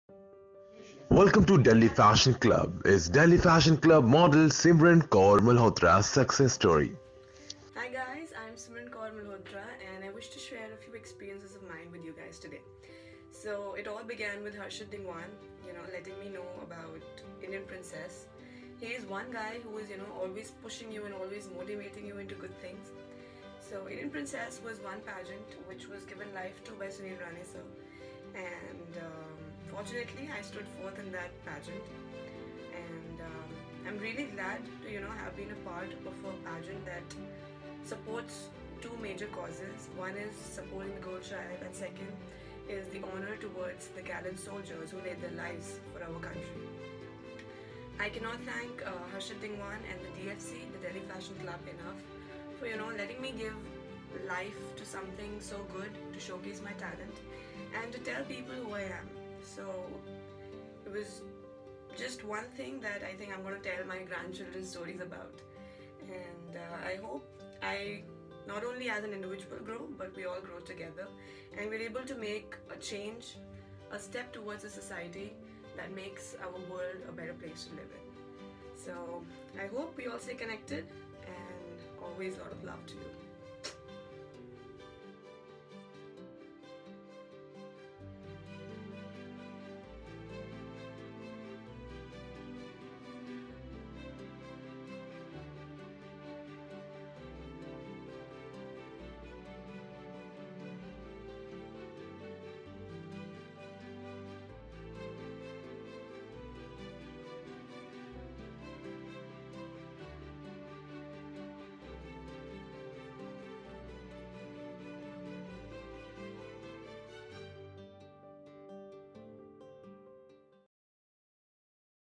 DFC Model Success Story | Simran Kaur | Groomed by Harshit Dhingaun | Delhi fashion club
Simran Kaur Malhotra won top 5 Finalist place at Indian princess 2016, here she share experience with Delhi fashion club and Harshit Dhingaun.